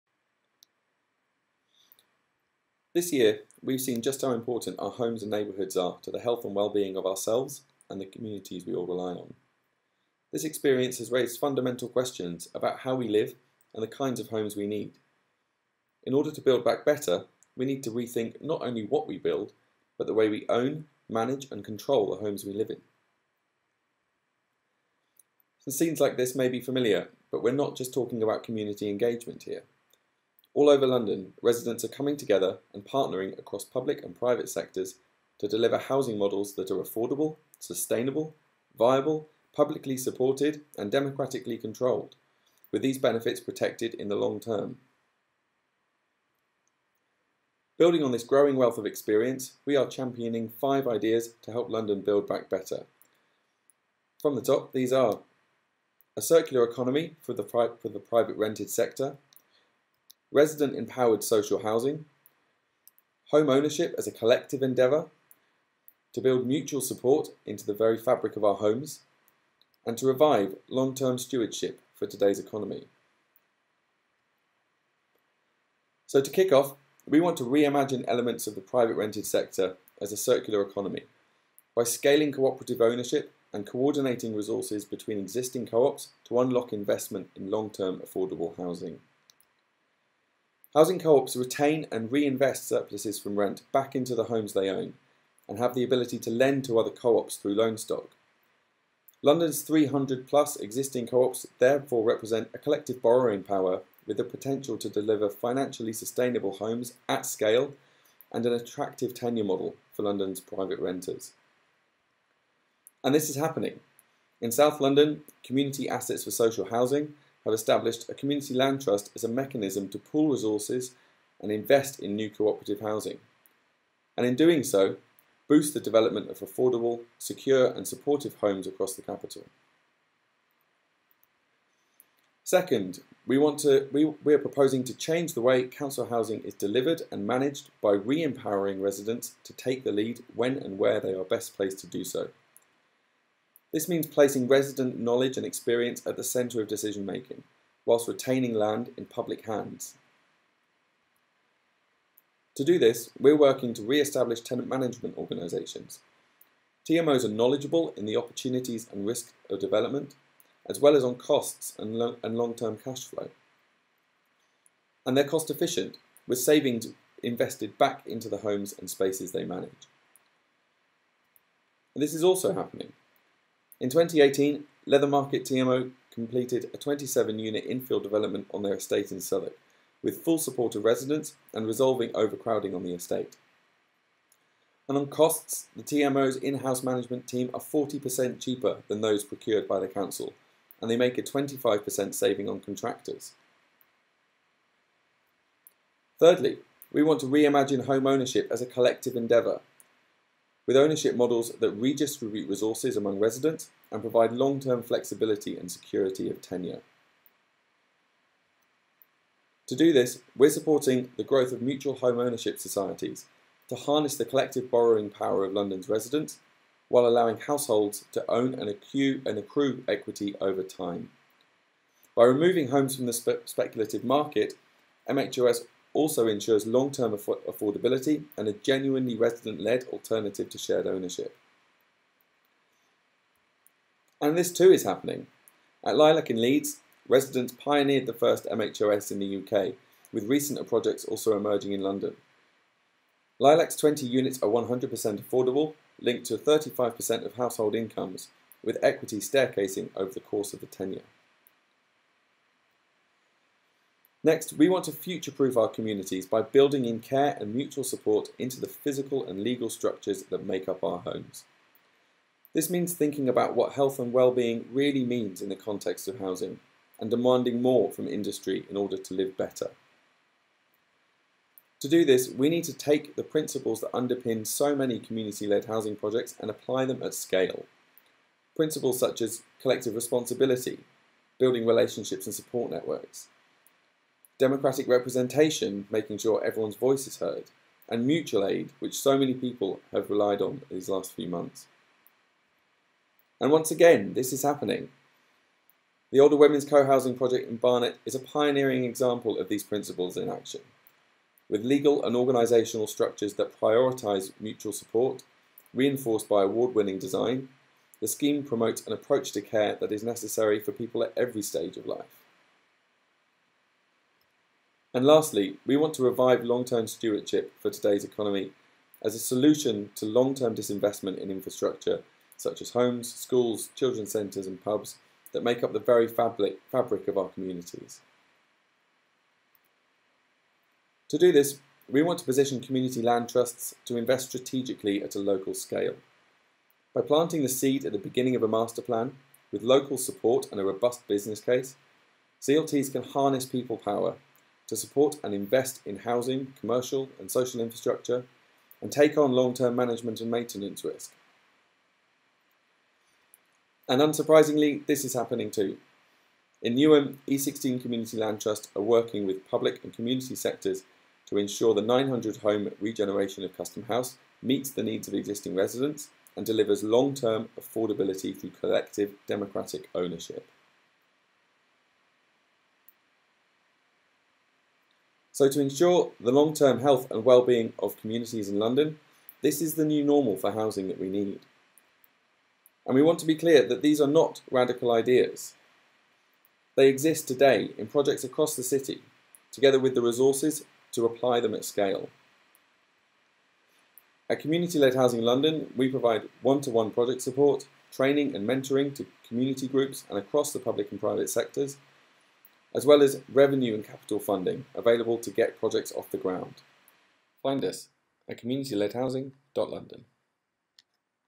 Build Back Better - Rowan Mackay presentation
Ideas and inspiration for how community led housing can offer solutions for current times.